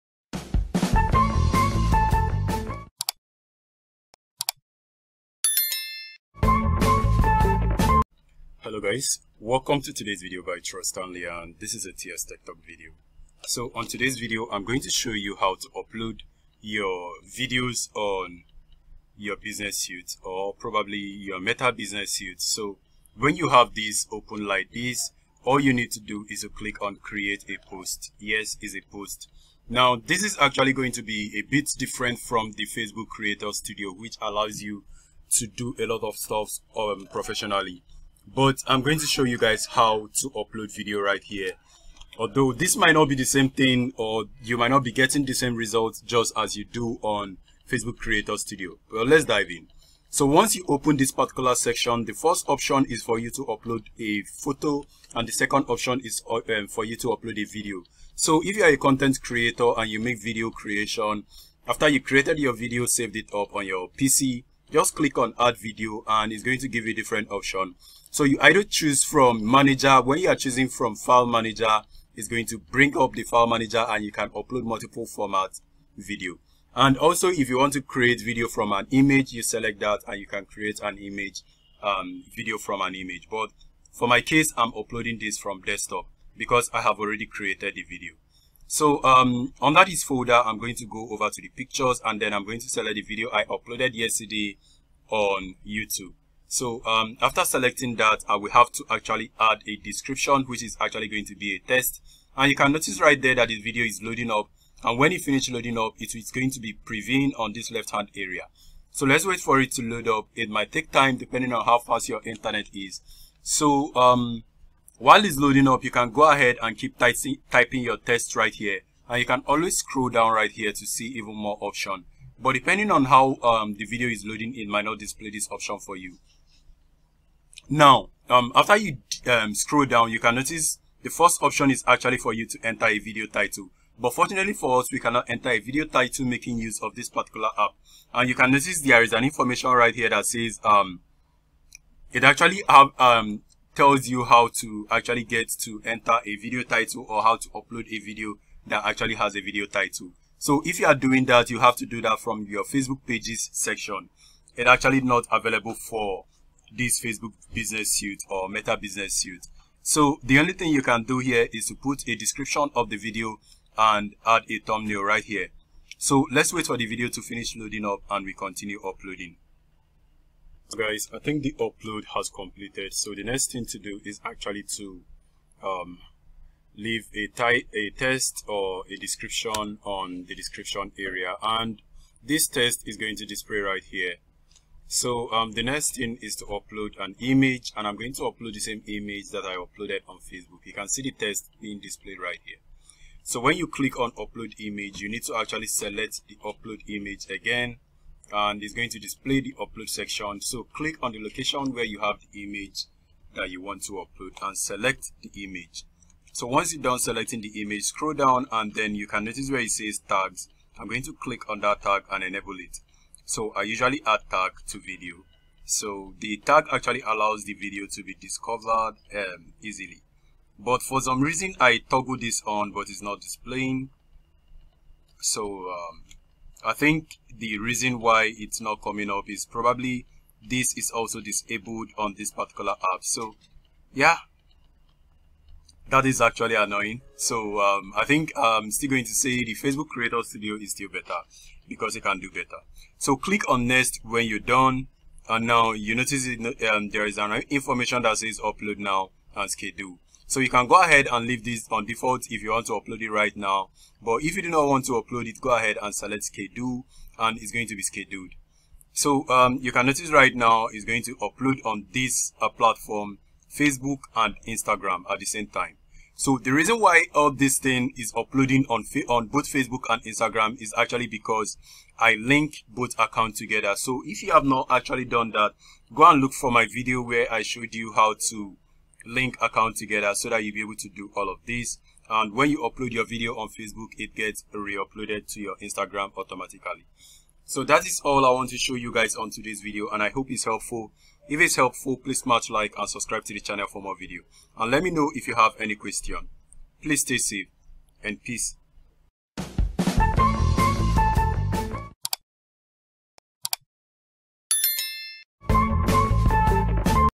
How To Upload Video To Facebook Business Suite
How To Upload Video To Facebook Business Suite, also called Meta Business suite and i will show you how to upload video wit thumbnail and description although this is not same way as we upload on Facebook creator suite.